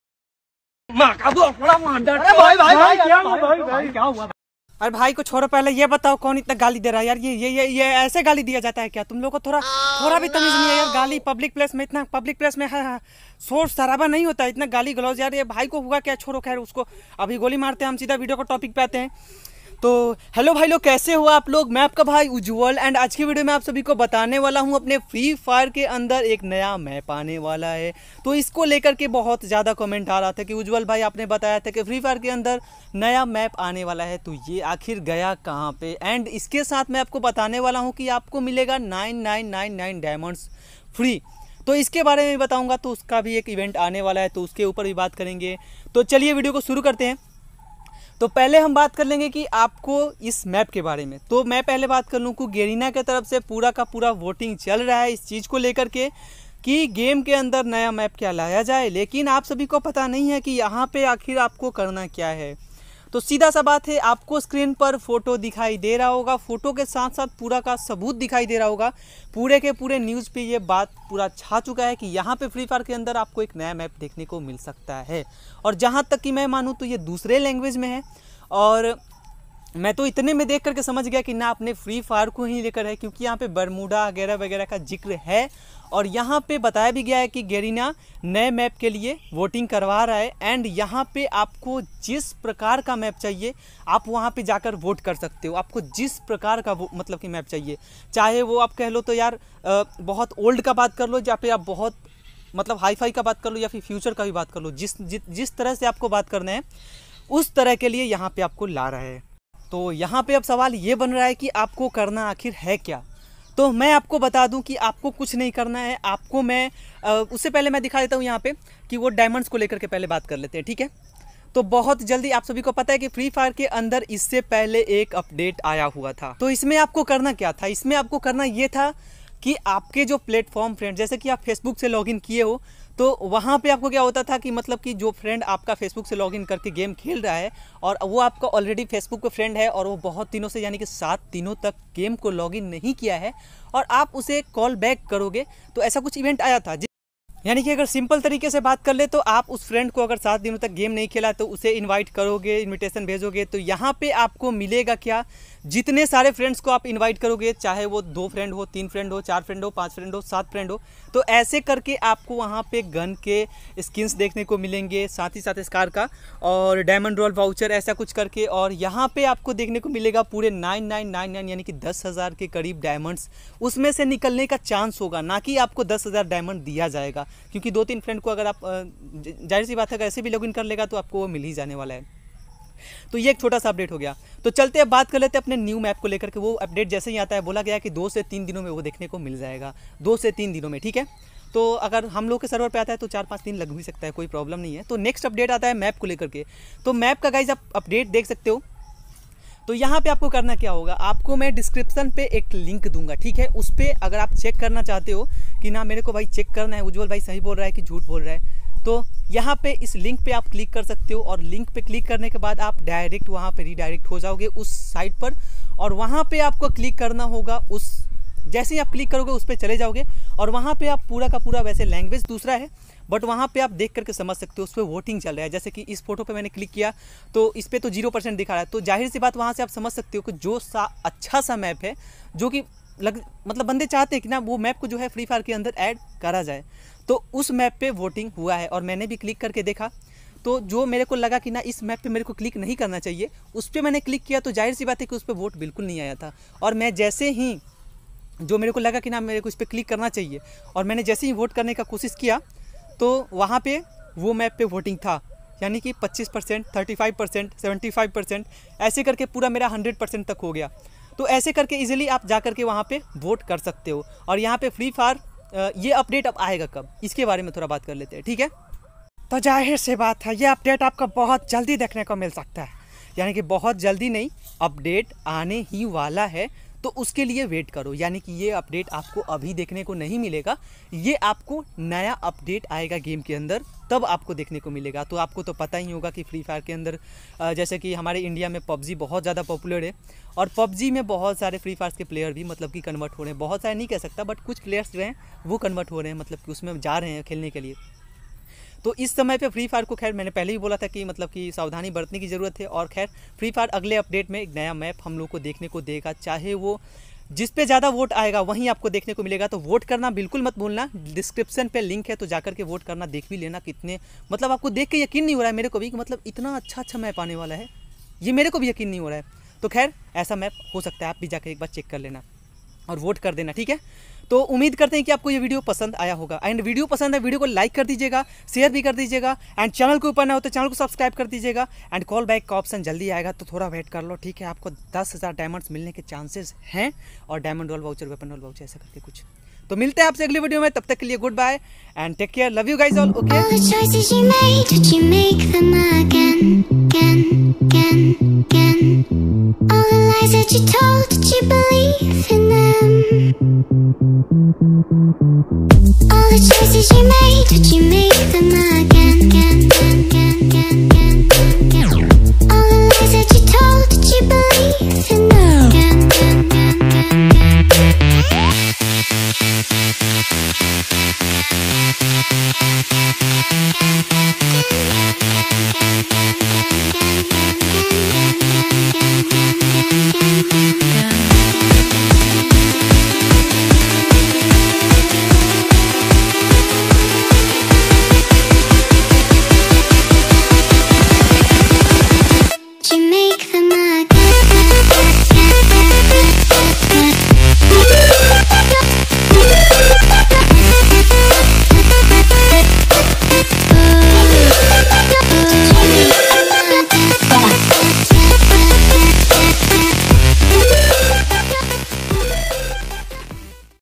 Free 9999+💎💎 Diamonds Call Back Event Full Details, New Map Go And Vote☑
Call Back Event Is Back Free 9999+💎💎 Diamonds, New Map Go Fast And Vote☑

2nd* channel 👇😘

game id- 👇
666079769

Guild I'd- 👇
65416792

So...i hope aapko pasand aaya hoga... 😘😂😍❤💕❣

unique_gameplay

Ģűd_Đay & Tāke Çarè 🥰🥰